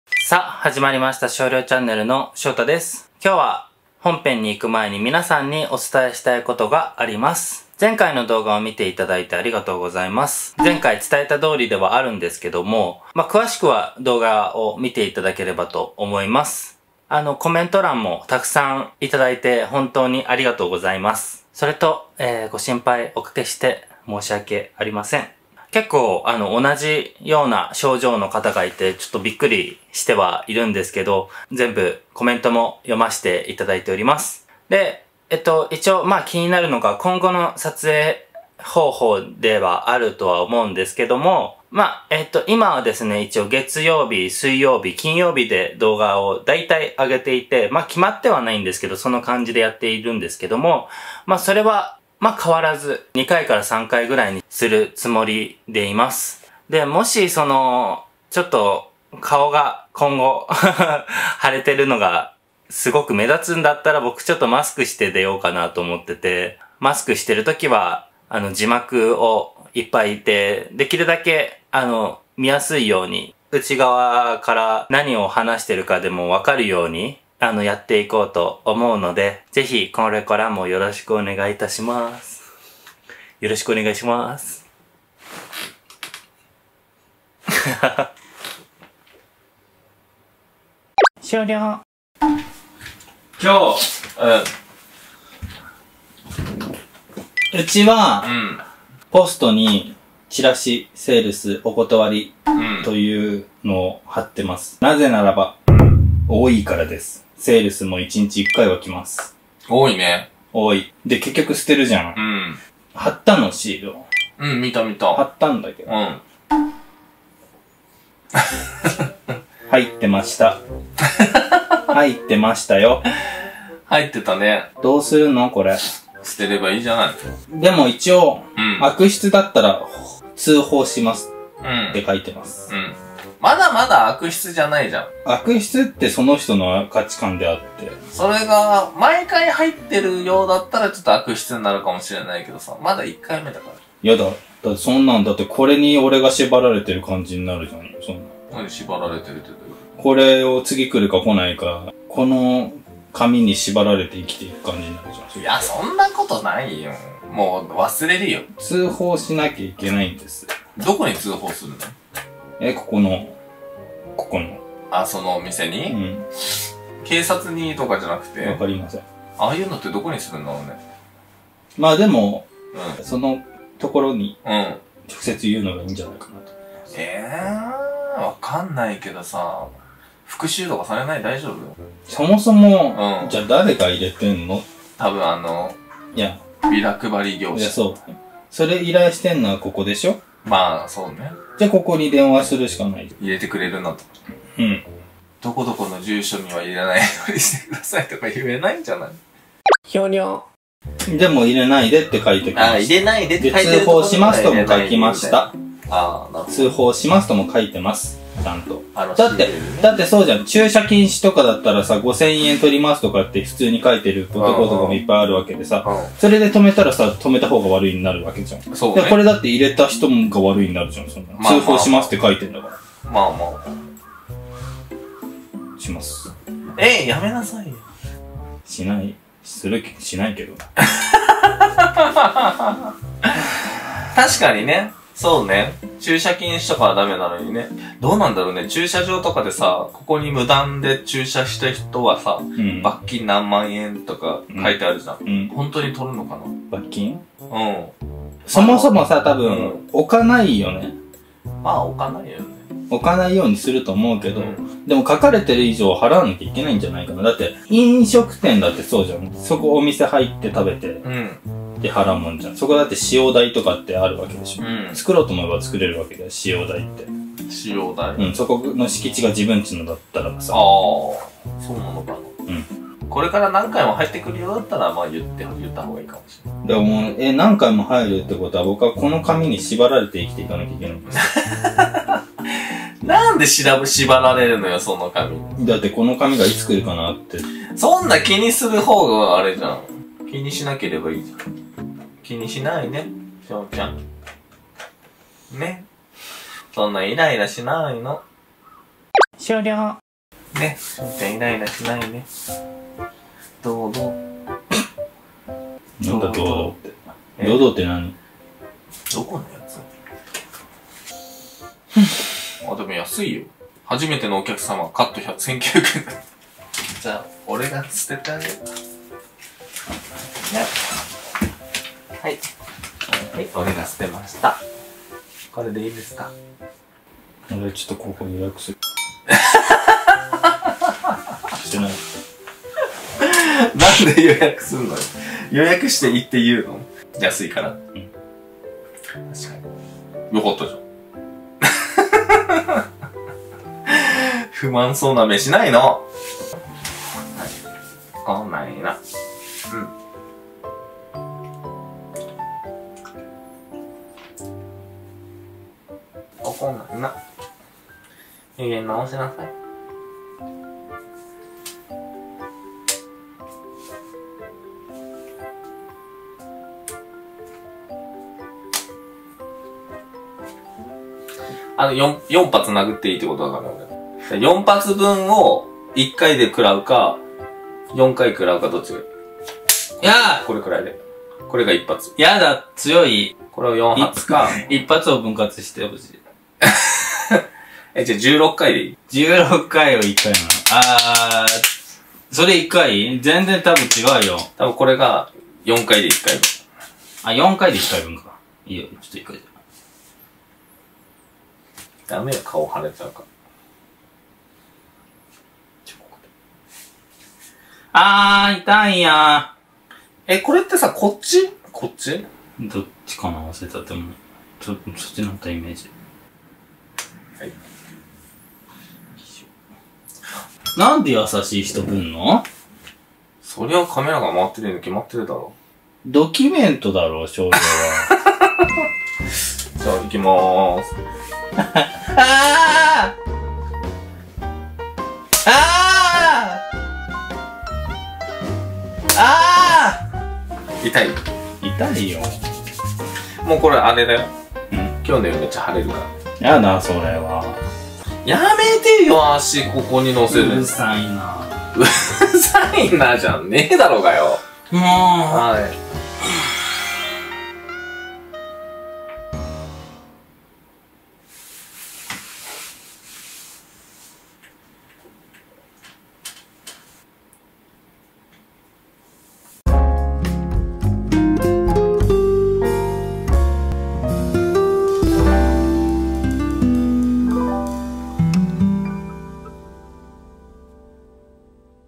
SUB)【ゲイカップル】警察に通報？朝から年下彼氏が怒ってる理由... /BL
セールス チラシ 勧誘 一切お断り 悪質な場合 即刻通報 耐候 防水 高級アルミヘアライン ステッカー 145X30mm(横 1枚, アルミヘアライン)

・結婚記念日の動画
・しょーた(年下彼氏)について
・りょう(年上彼氏)について
今まで付き合った人数は？？